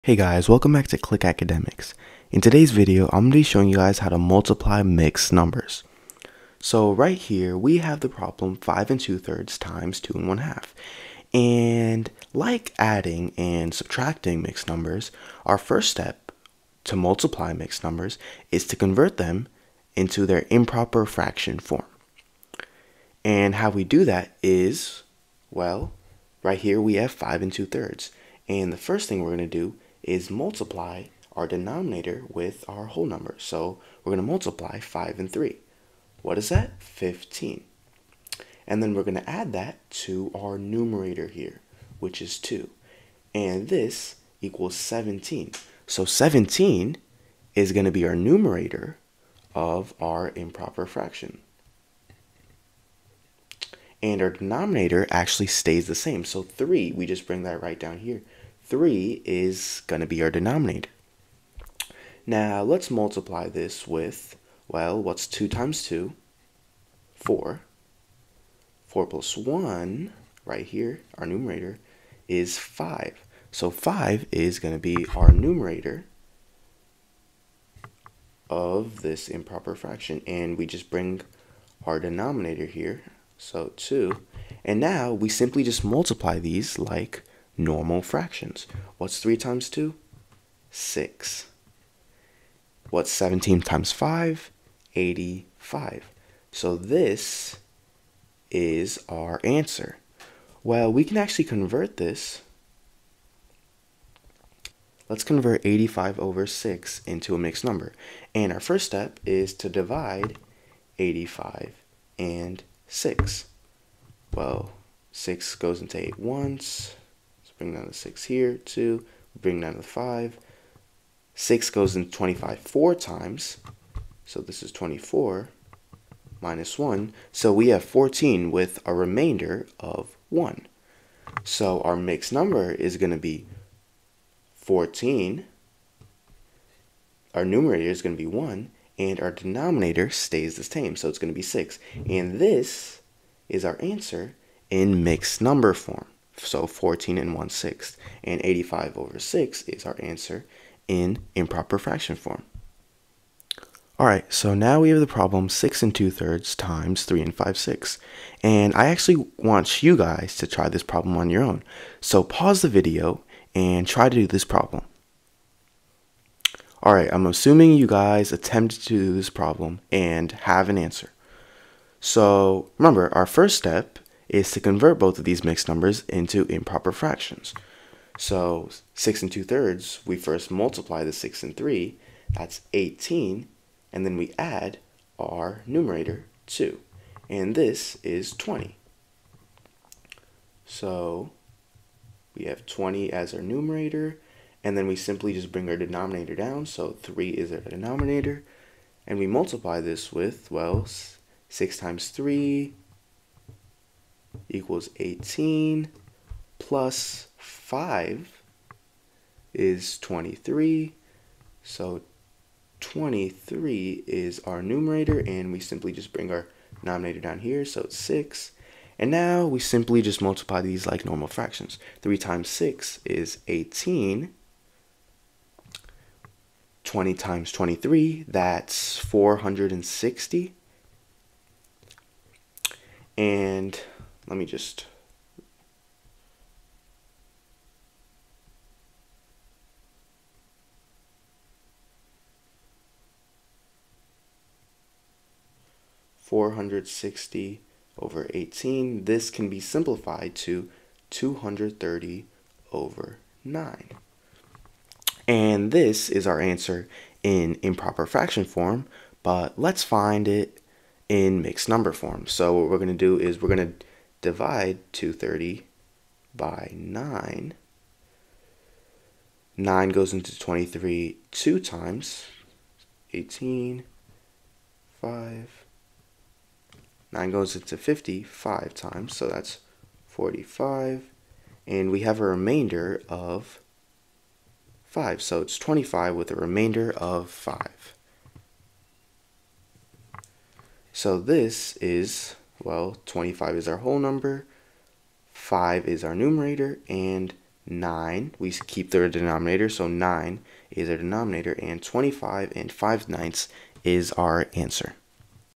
Multiplying Mixed Numbers
In todays video, we will be teaching you how to multiply mixed numbers.